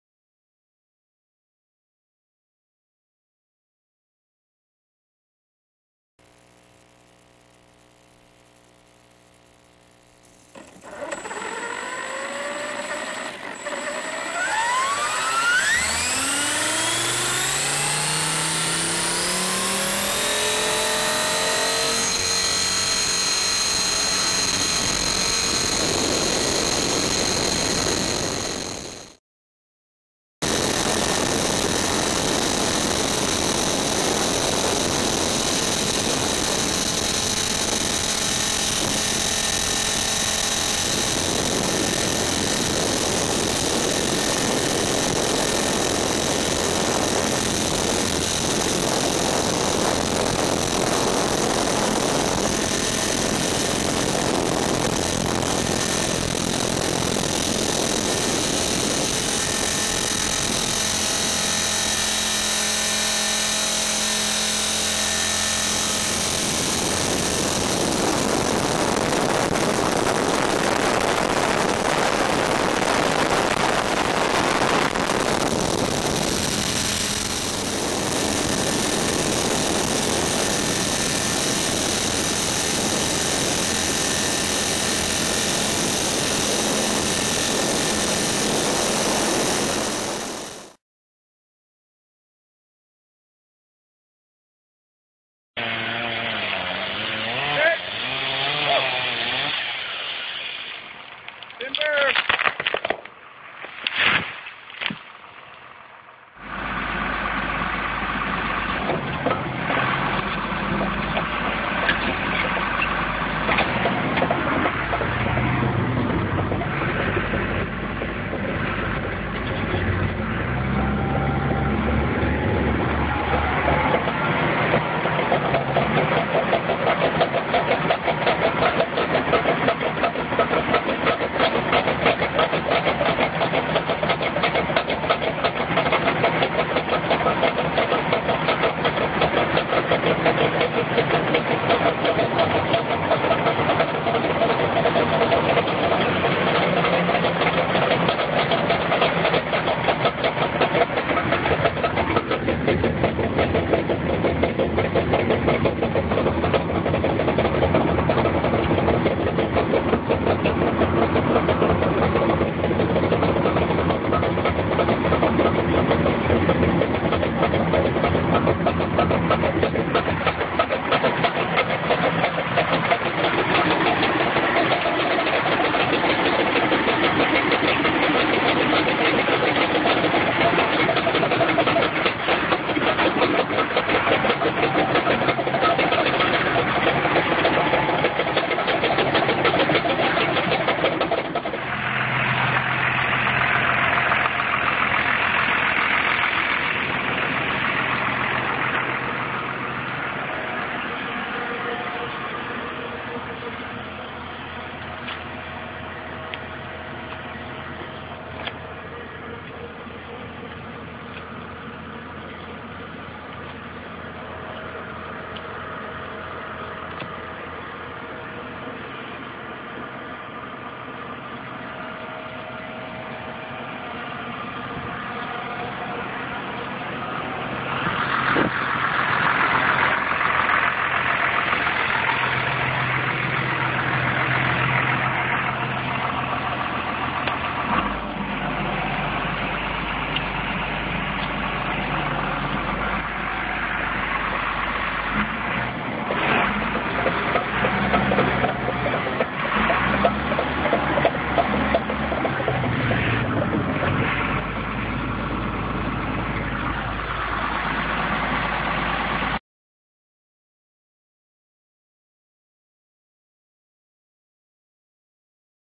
FPV of the Pine Tree
An FPV of a Pine Tree from Helicopters and Dozers perspective. I would think at this point it is one of the more unique videos on YouTube? If anyone has anything similar pleas send me a link.